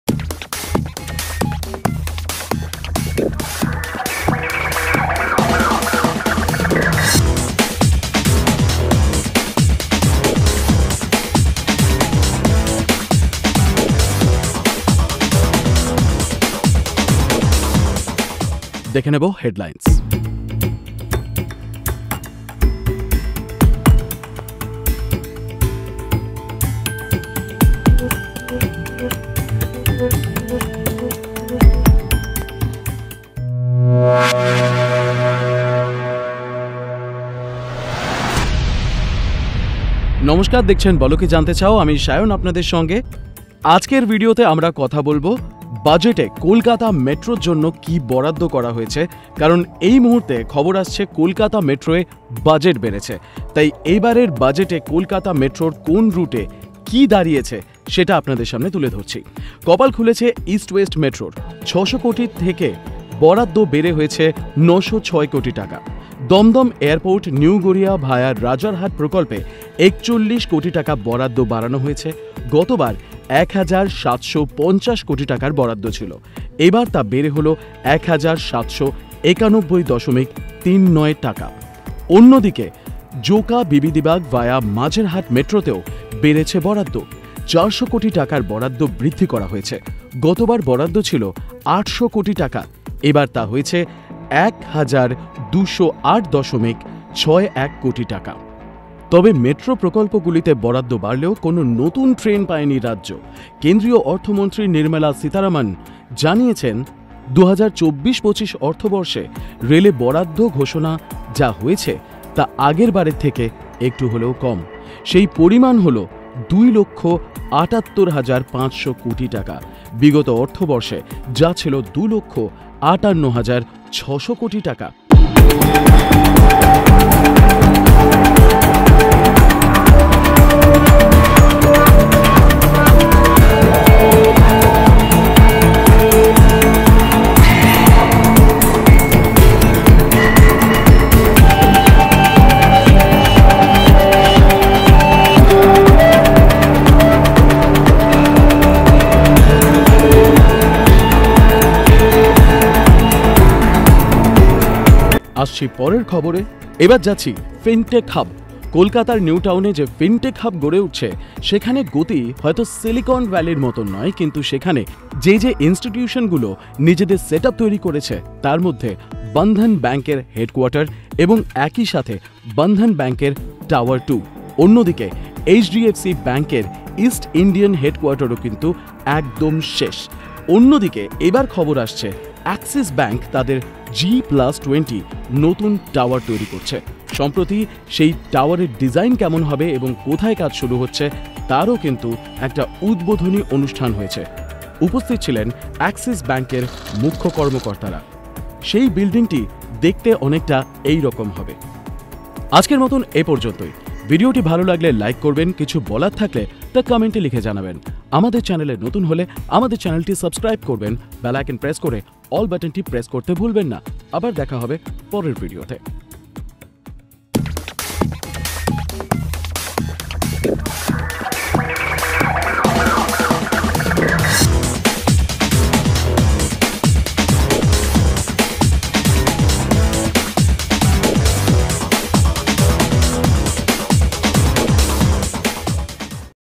KOLKATA METRO GOT MORE BUDGET FOR NEWGARIA AIRPORT & EAST WEST METRO ROUTE | AXIS BANK FINTECH HUB
KOLKATA METRO is the lifeline of the city of joy and the city's life line got huge budget. It would help riders and the city's economy. East west metro route, New Garia to Kolkata airport metro route and Joka Esplanade metro route got the huge budget.
on the other hand Axis bank to start it's construction worker at Fintech Hub Newtown Kolkata where already Bandhan Bank, HDFC and National insurance company and SBI made their presence.